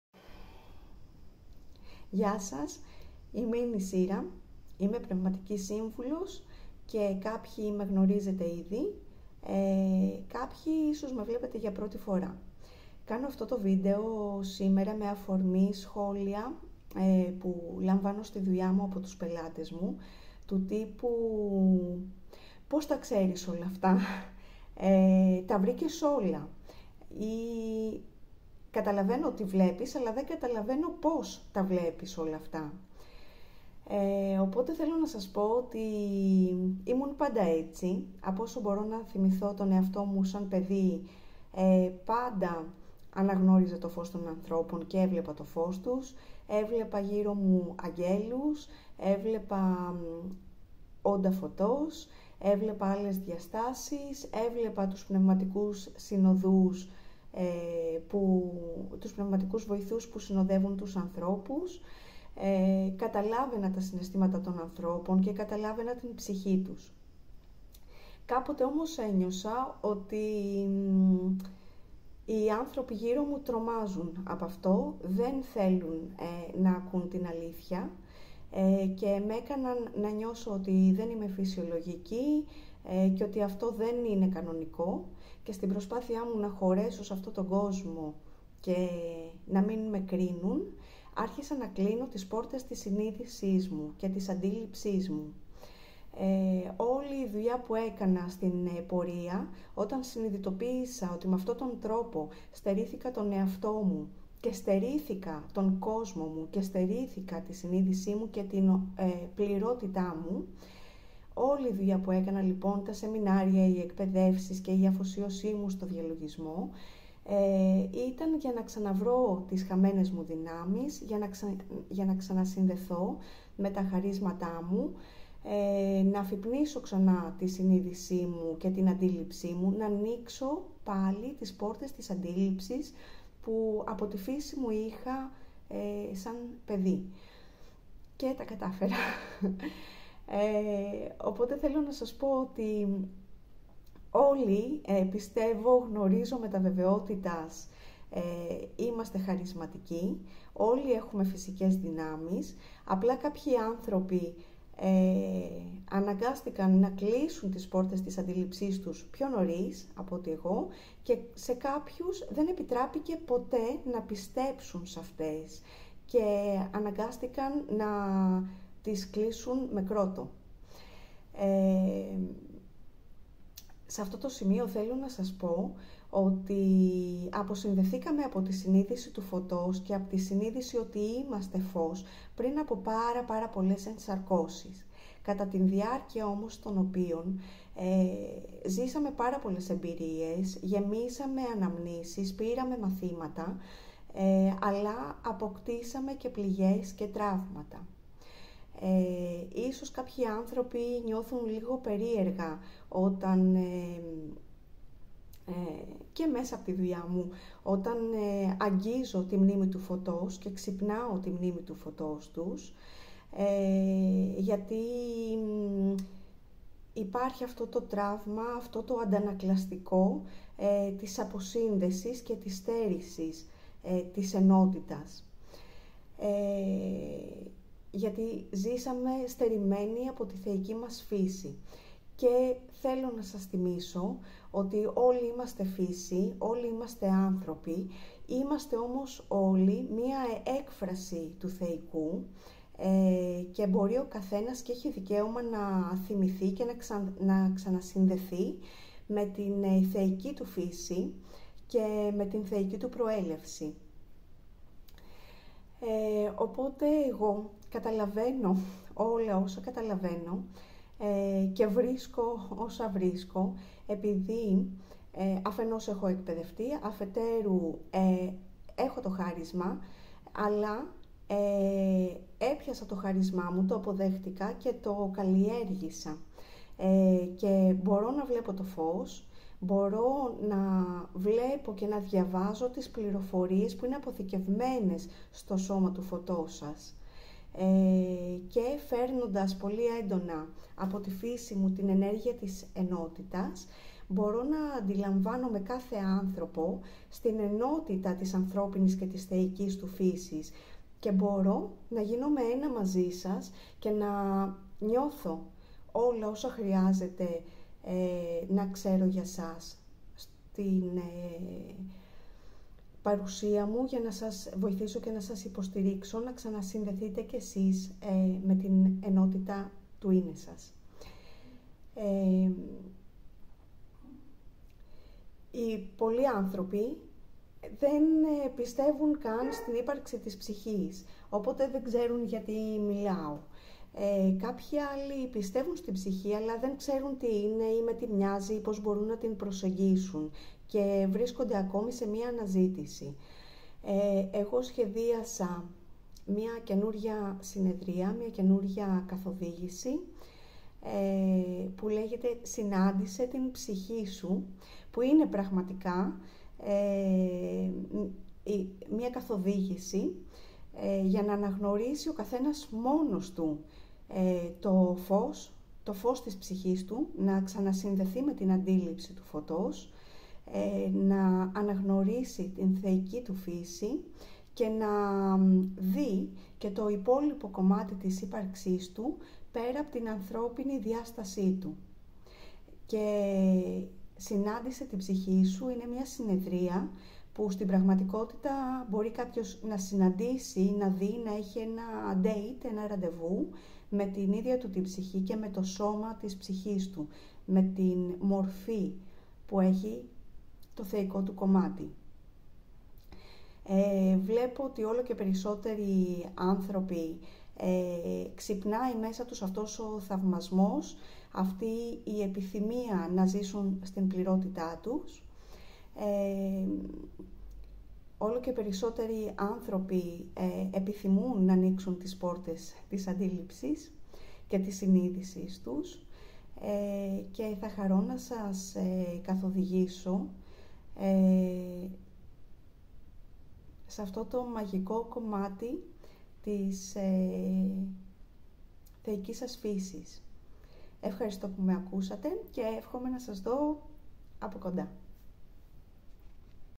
Η Μνήμη του Φωτός. Συνάντησε την Ψυχή σου | Νισίρα Λουκά | Heart of Love Lightcenter
Η Μνήμη του Φωτός και η Συνείδηση ότι είμαστε Όντα Φωτός, οι πόρτες της Αντίληψης και η Ενότητα της ανθρώπινης και της θεϊκής μας φύσης.
"Συνάντησε την Ψυχή σου" μια συνεδρία για να αναγνωρίσεις την θεϊκή σου φύση και να δεις την μορφή της ψυχής σου.

Νισίρα Λουκά,
Πνευματική Σύμβουλος, Θεραπεύτρια, Εκπαιδεύτρια
fb Nisira Louka